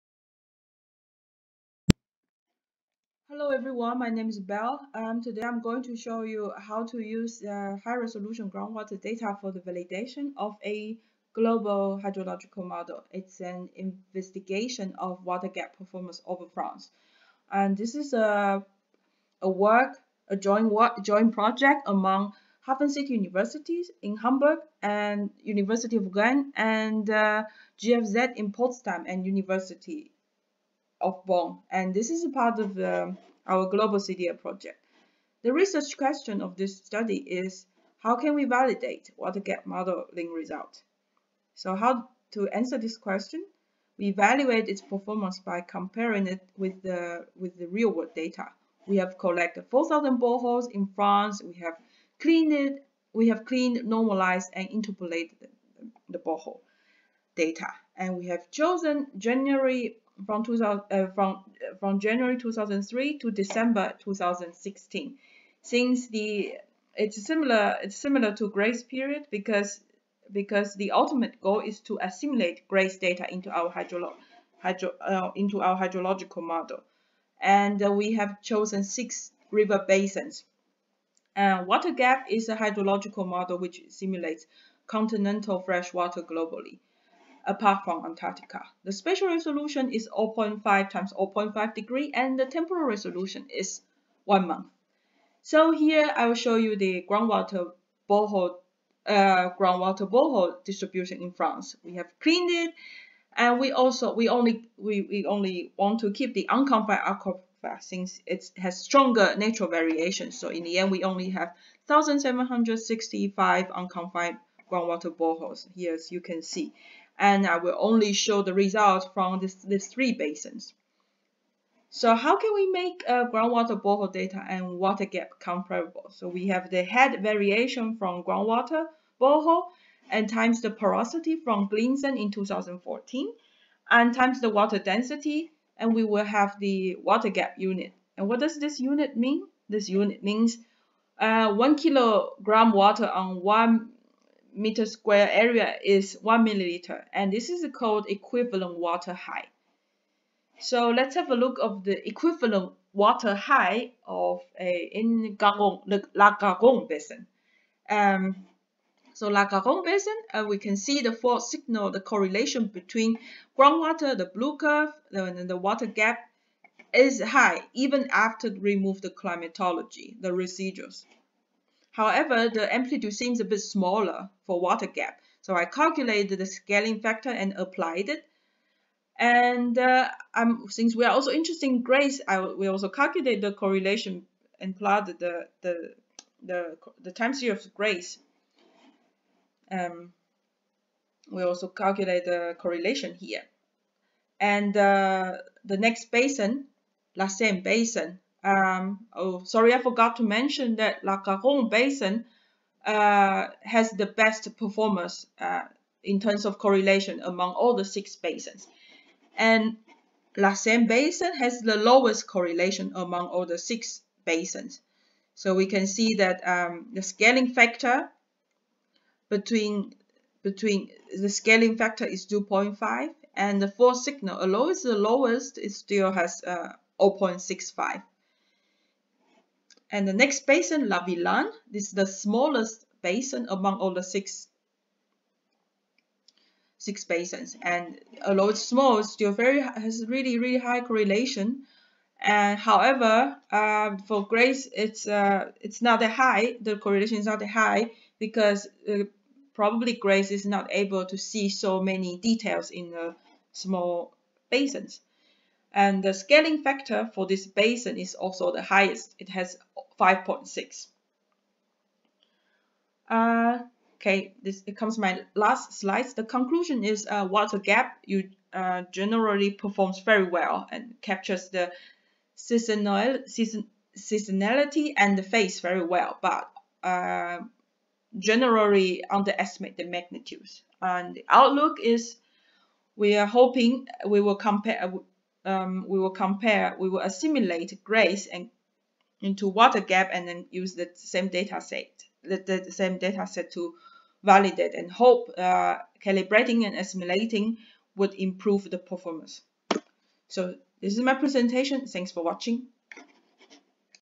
118 Using High-resolution Groundwater Data for the Validation of a Global Hydrological Model
Kuei-Hua Hsu, HafenCity University, Geodesy and Geoinformatics, Hamburg, Germany